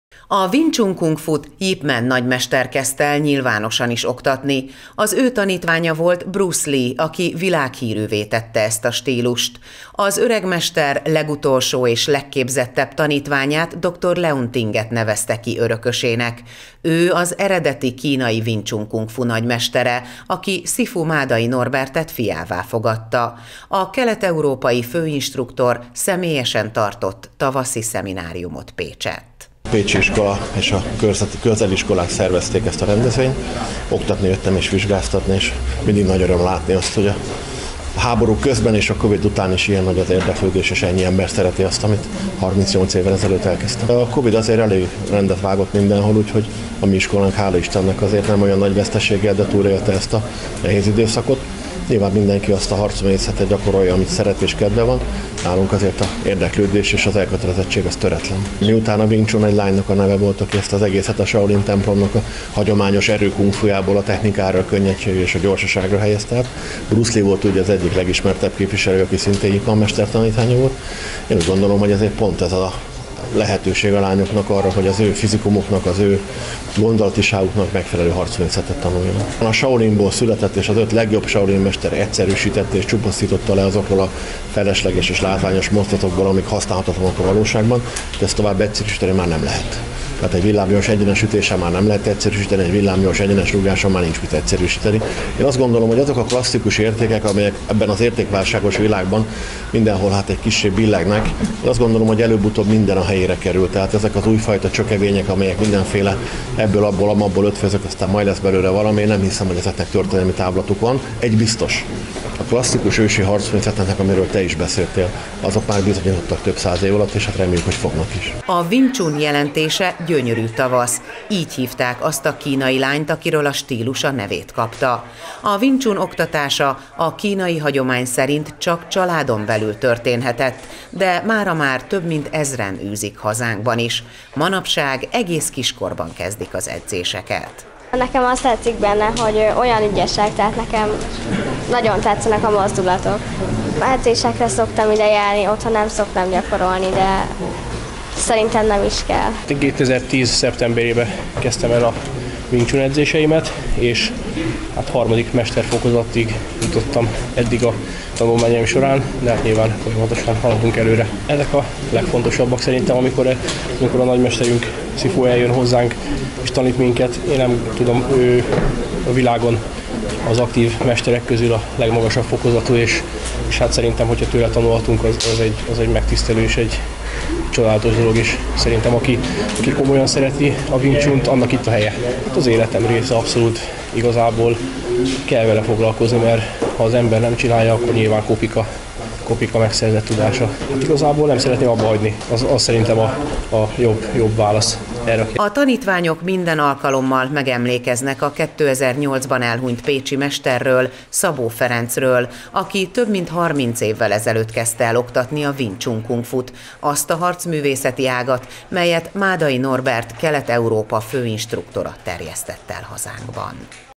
Wing Tsun Kung Fu szeminárium Pécsett
A Wing Tsun Kung Fu stílust Bruce Lee tette világhírűvé. A harcművészeti ág eredete a Shaolin kolostorig nyúlik vissza. A középkori Kínában az akkori idők legjobb shaolin harcosnője fejlesztette ki nők számára. Koncepciója és technikája gyökeresen különbözik a hagyományos harcművészetek felfogásától. Április utolsó napjaiban SiFu Máday Norbert tartott szemináriumot Pécsett.